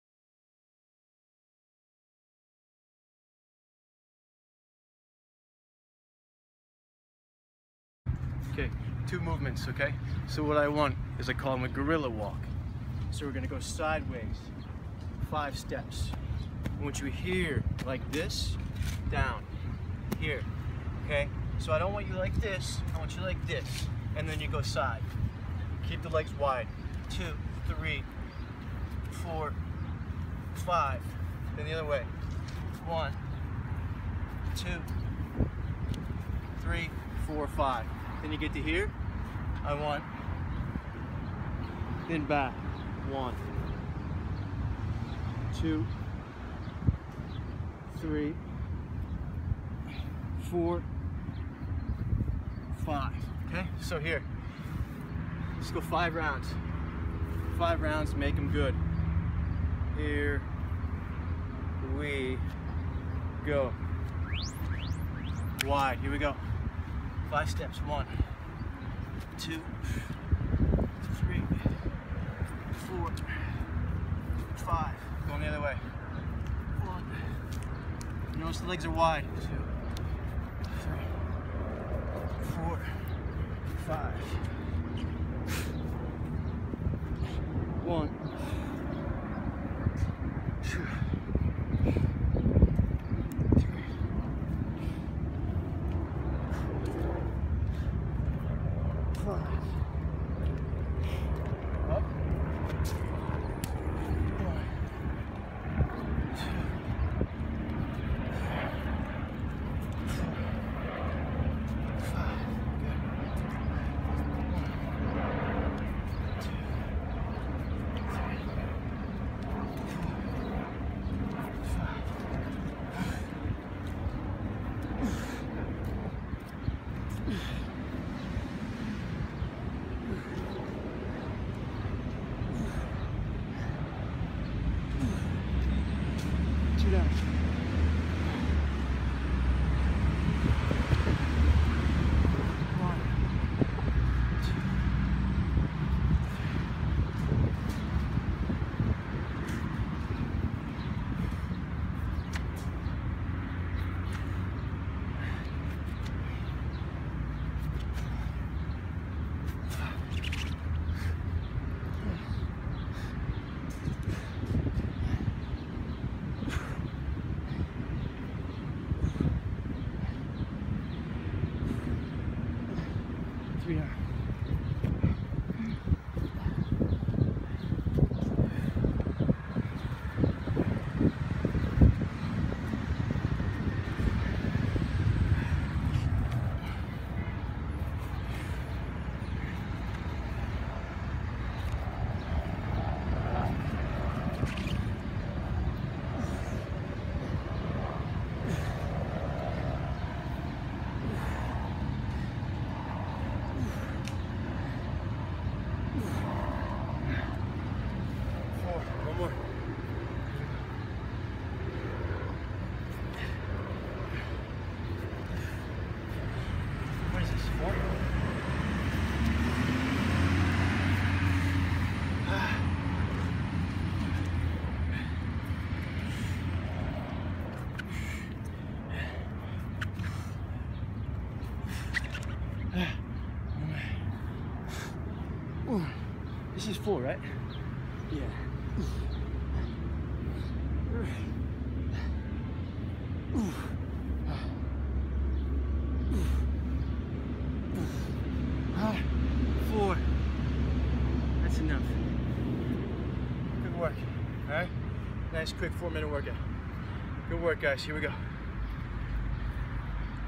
Gorilla Walks & Facedown Extensions
In this video, Jayson Westley is going to show you two different core fitness exercises- gorilla walk and face down press and extend. These full body exercises are very helpful to improve your co-ordination and mobility.

To get started with gorilla walks, first   Place your hands on the ground and out to one side about under your shoulders. Move your weight forward onto your hands and press off your hands to enable you to hop your feet over to one side. When you will be landed, continue doing that gorilla walk both of your right and left sides for 5 reps for each side for 5 rounds.

Now to get started with face down press and extend, first lay down on the ground on the weight of your chest, now, straight your head and straight out both of your hands forward and make a fist shape and then bring your hands backward. Continue doing this for 5 reps for 5 rounds.

You can use social media for the latest news and updates on content from jayson westley! We'd appreciate if you'd: